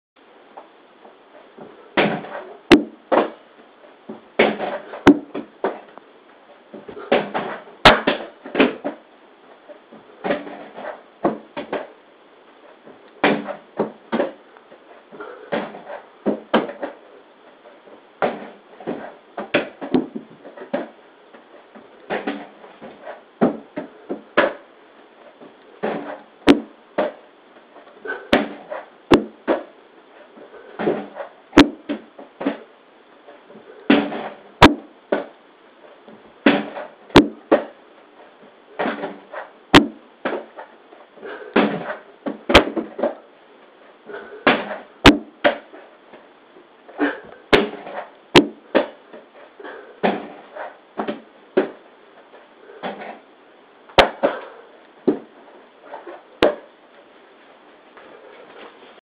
超シュール！おっさんの玉入れ
長期に渡る年末年始のお休み2日目。
とりあえず自宅にあるバスケのゴールでシュート練習してました。
画がシュールと言うか地味です。
チビッコ諸君。バスケやろうぜ！